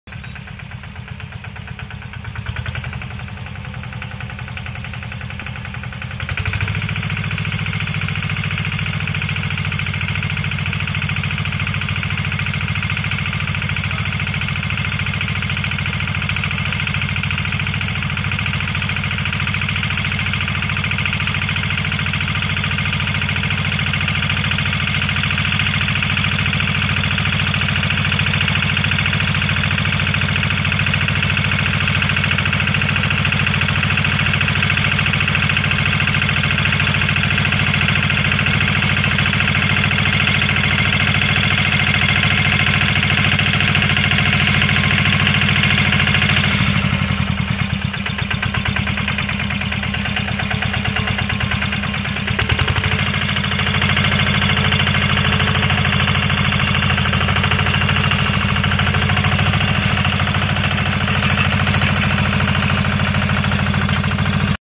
Aardappels planten Ferrari 72s. Planting potatoes with Ferrari 72s. Semina patates con Ferrari 72s
Aardappels planten met de Ferrari 72s op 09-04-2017.

Planting potatoes with Ferrari 72s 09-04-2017

Semina patates con Ferrari 72s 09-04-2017